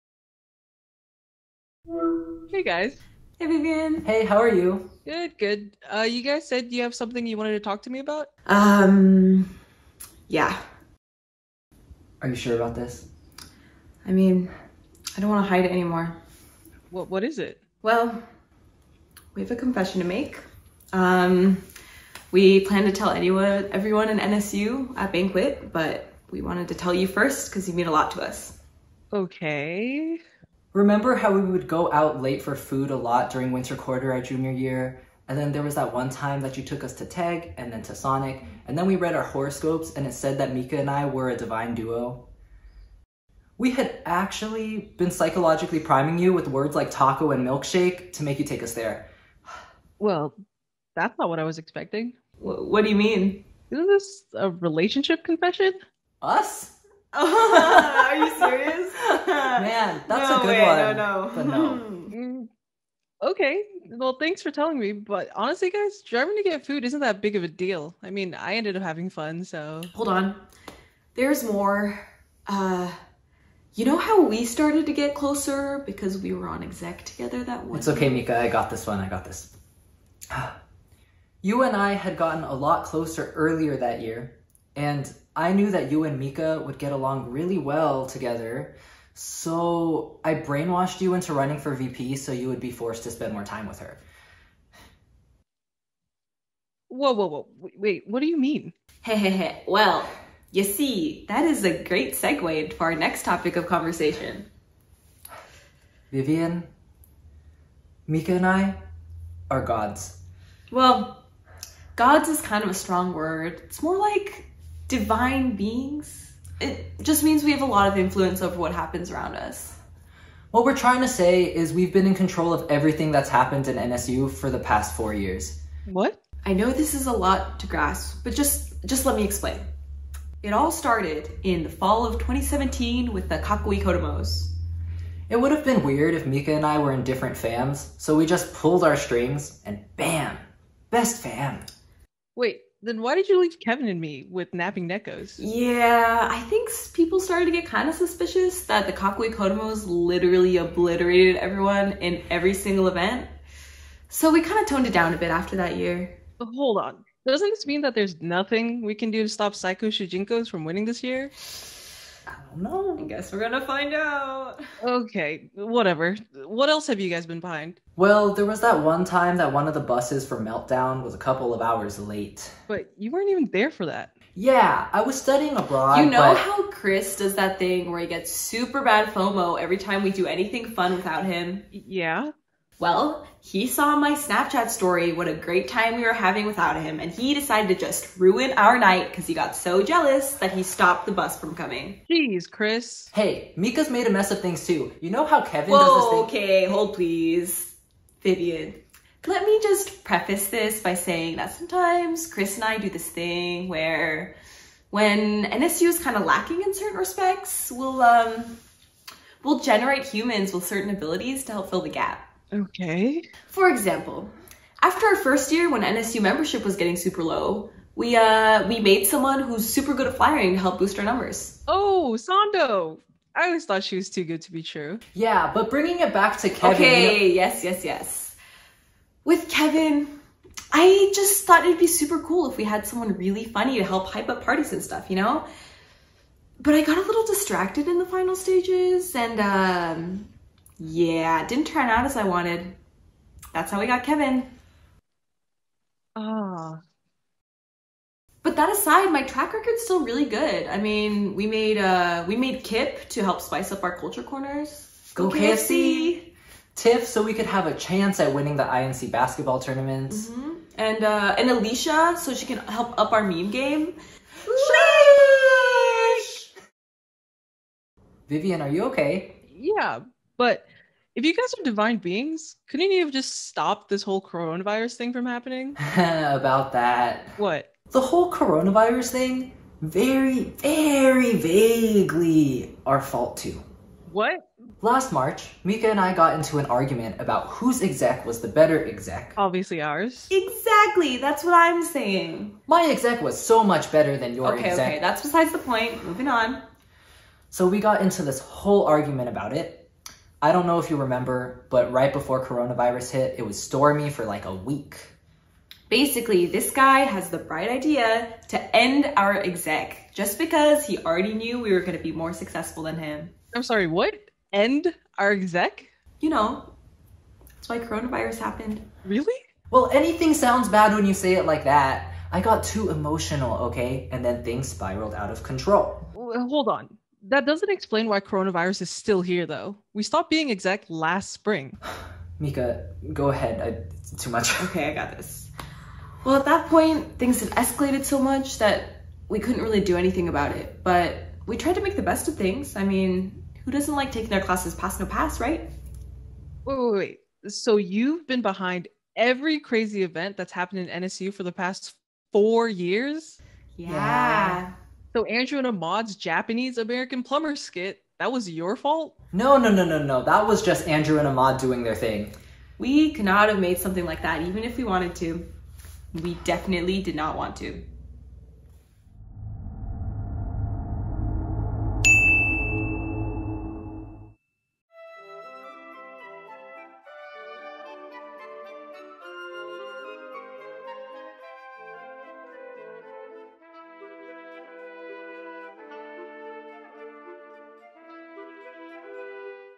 We Have a Confession... (Saikou Shujinkous) - UCSD NSU 2020-2021 Fam Heads
Thanks for 4 wonderful years Nikkei Student Union. It has been an honor to work alongside and get to know all of you throughout my time at UCSD. Mika, Vivian and I thought this would be a fun way to end the year by letting everyone reminisce and chuckle a little bit.

Even if Saikou Shujinkous don't win this year, I hope that all our fam members had a. fantastic time despite having a virtual year, and they will remember that they are and always will be the BEST!

Thanks again to Sophia (Mika) McDaniel and Vivian Do for taking part in this silly sketch, and if you want to see. the original Japanese American Plumber Skit, here's the link: